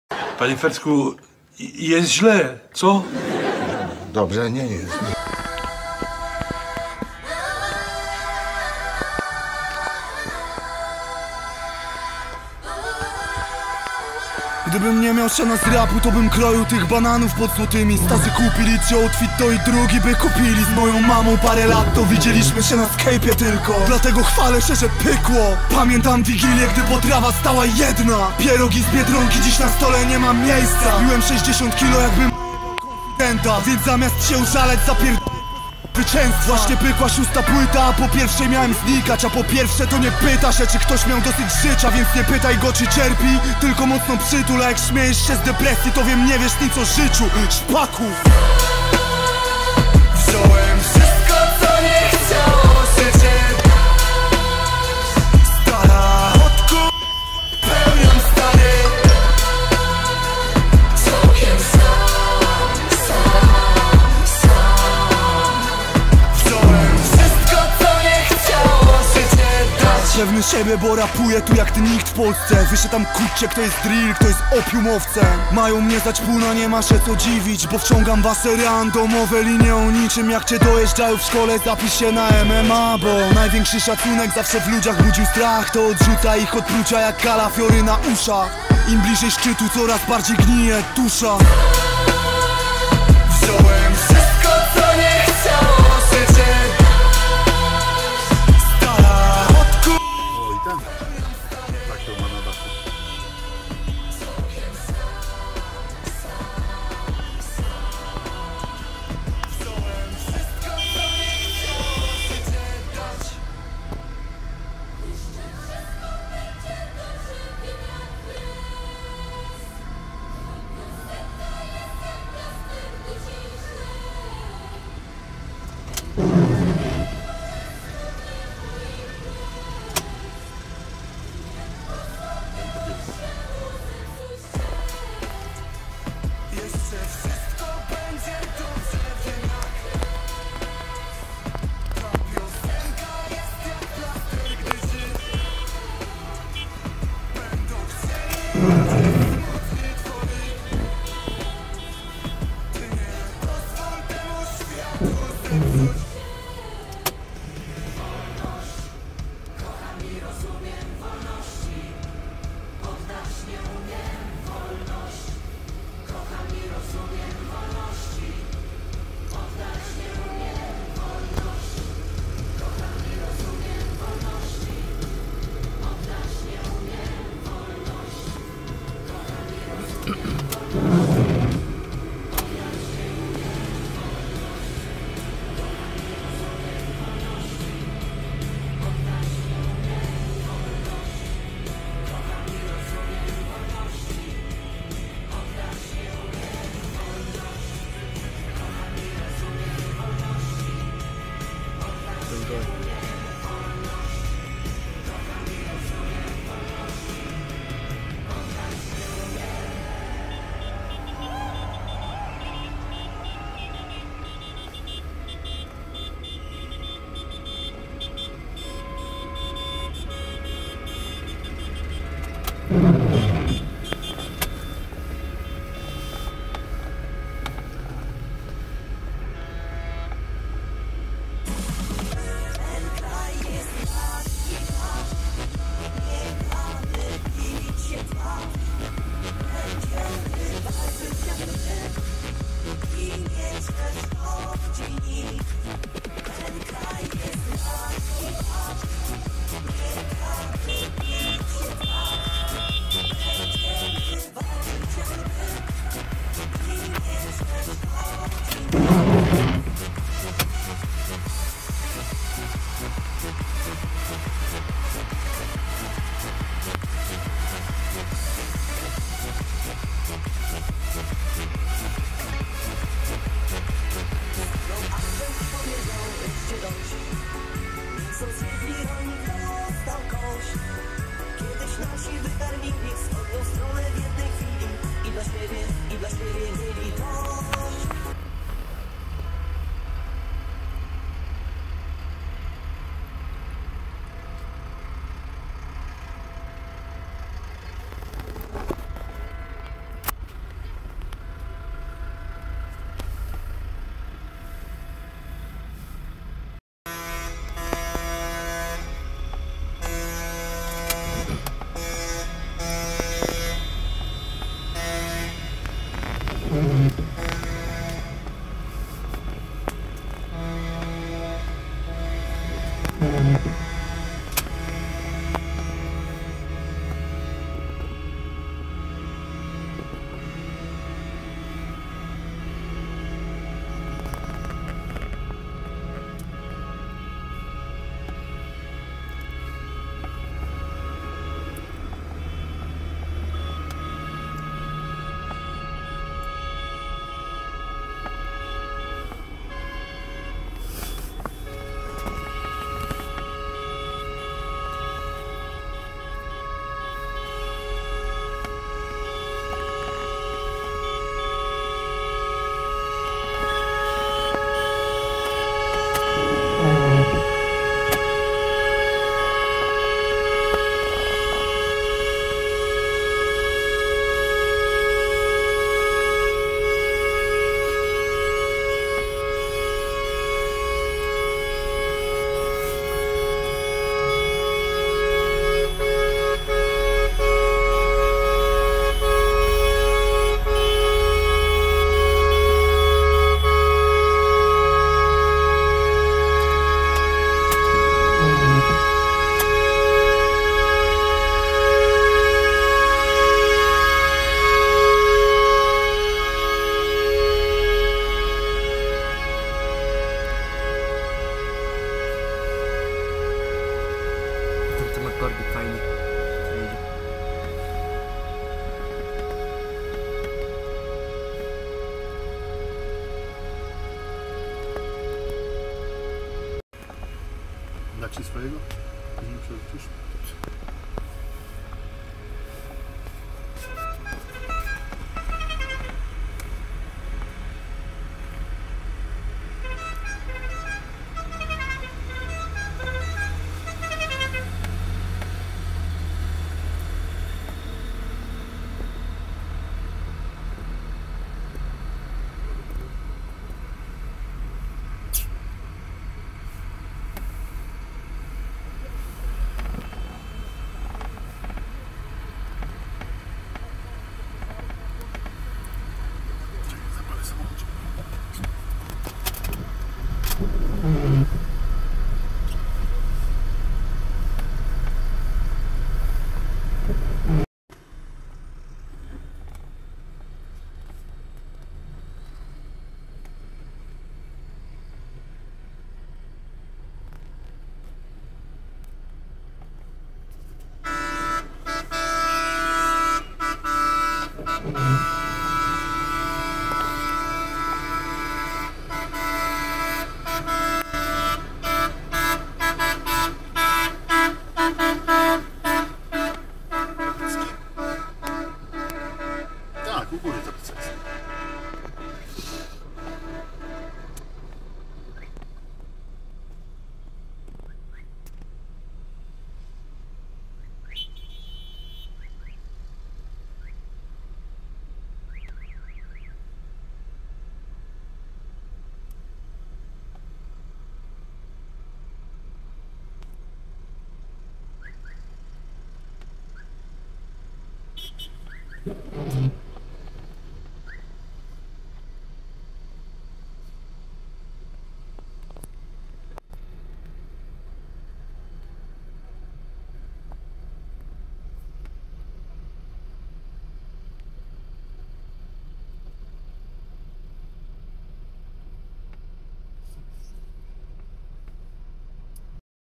🇵🇱 Strajk Rolników💥 Ryki 09.02.2024r Walczymy o swoje
W dzisiejszym odcinku strajk rolników który odbył się w Rykach
Walczymy o swoje